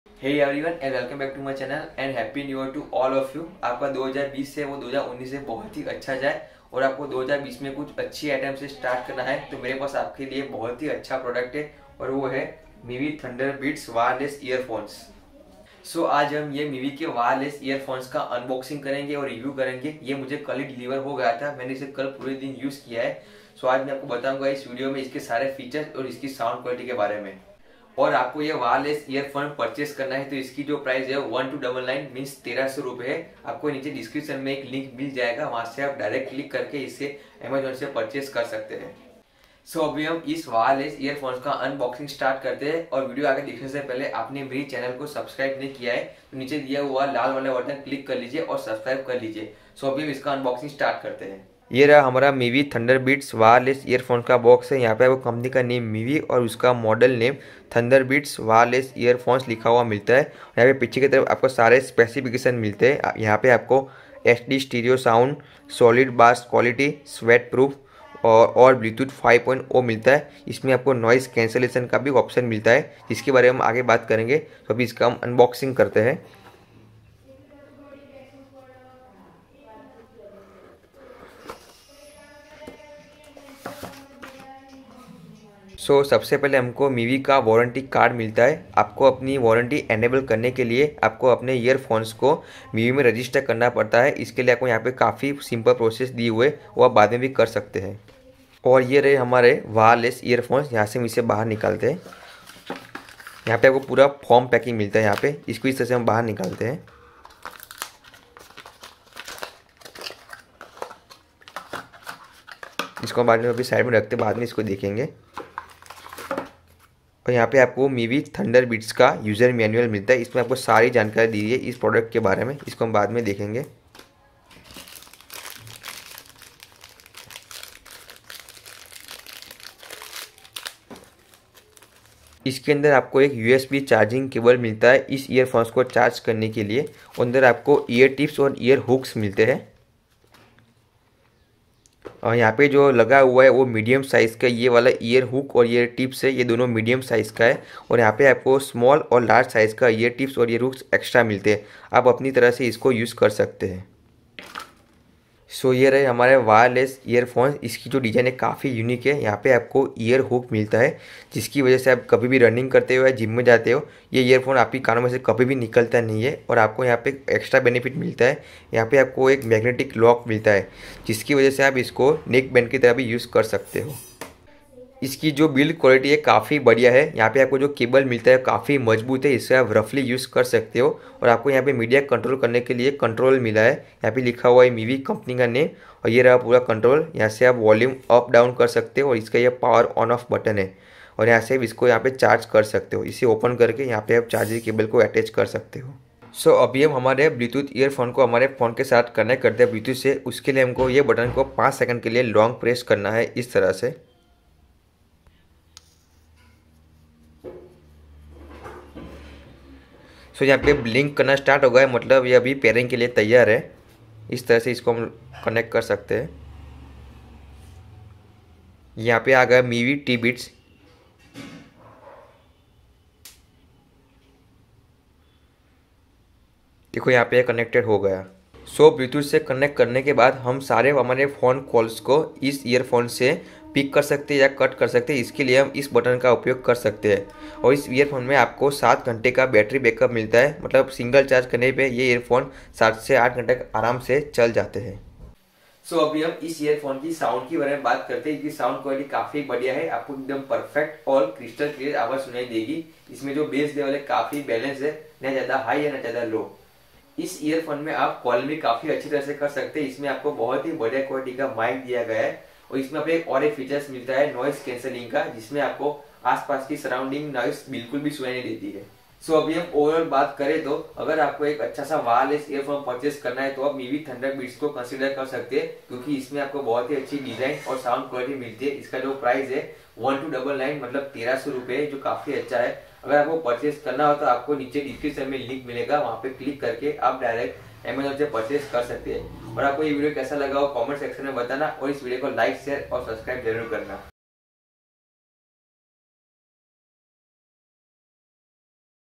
Mivi ThunderBeats Wireless Earphones full Review 2020 | Best Budget Wireless Earphones of 2020
Easy connect and extensive range. With Bluetooth 5.0, enjoy a 30 feet range of connectivity on these ear phones. 7 hrs of play time. Lithium-ion battery lets you enjoy up to seven hours of play time, rechargeable with USB power sources. Super bass and HD sound. The earphones boast of crisp HD sound, deep bass, clear mids and transparent highs. Stylish finish. Rugged, metallic exterior body made from premium materials that is sure to withstand most conditions. Magnetic lock. Magnetic lock helps to keep the Bluetooth earphones in place preventing them from falling when not in use. Hands-free wireless earphones with mic.